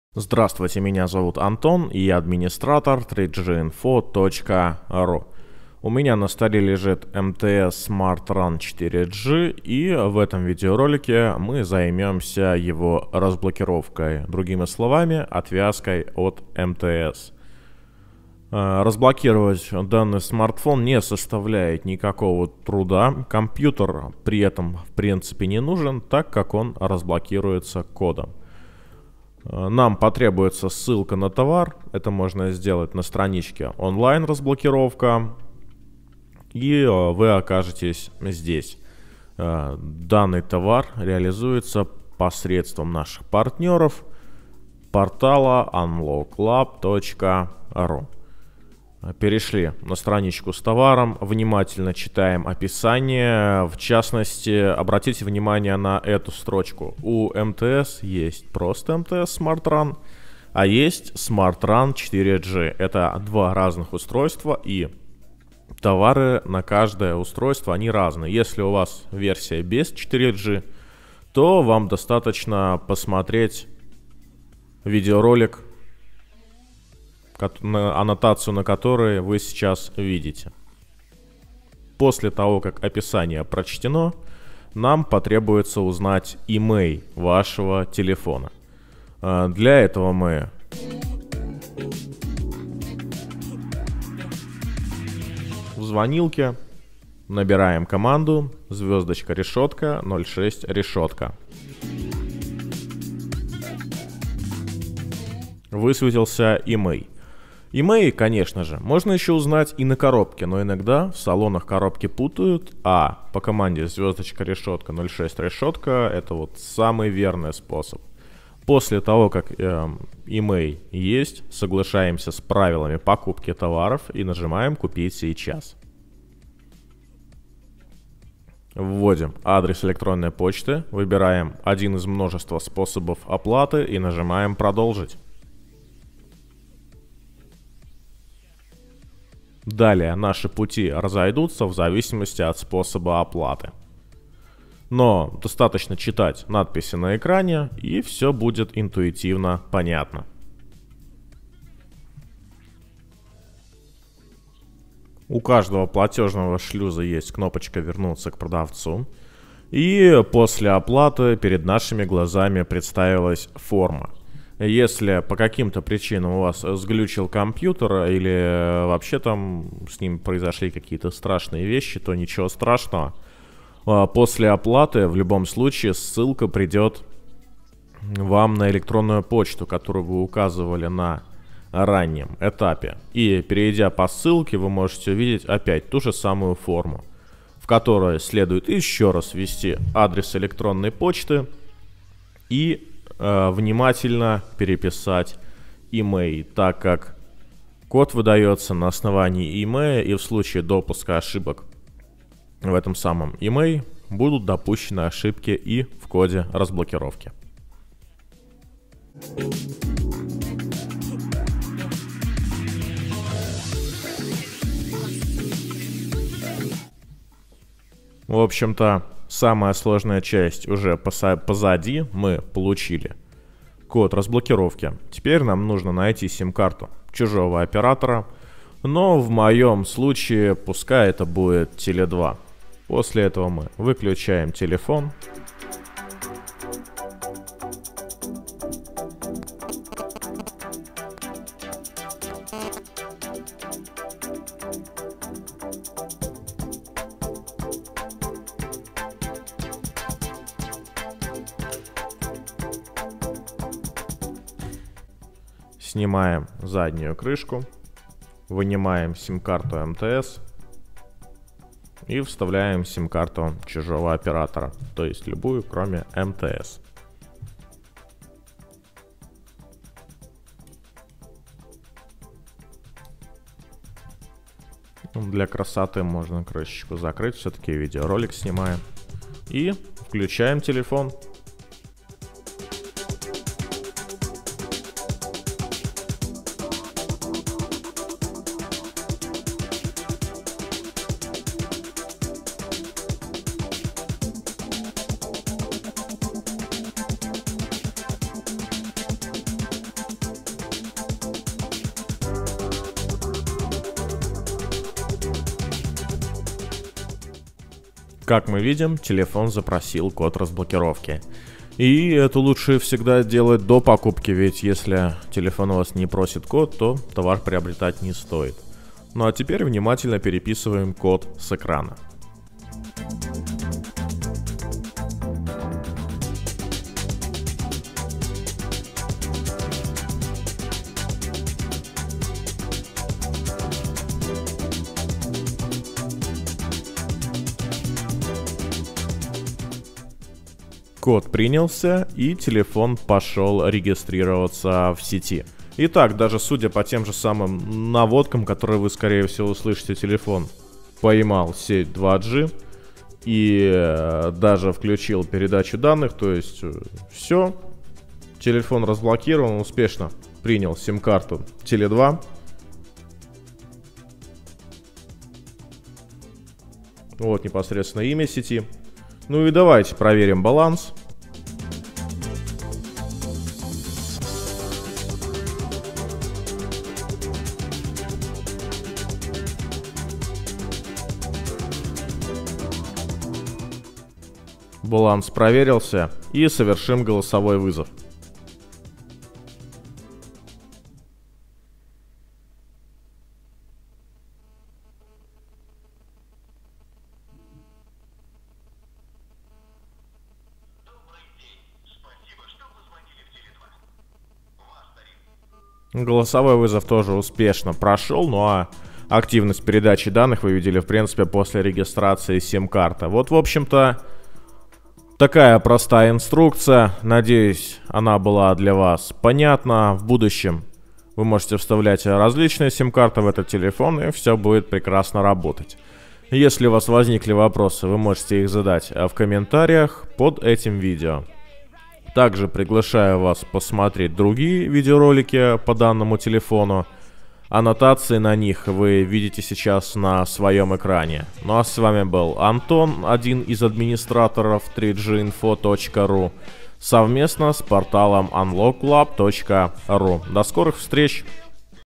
МТС Smart Run 4G. Разблокировка
В данном видеоролике показан процесс разблокировки от оператора МТС смартфона МТС Smart Run 4G.

Разблокировка телефона МТС Smart Run 4G. Код.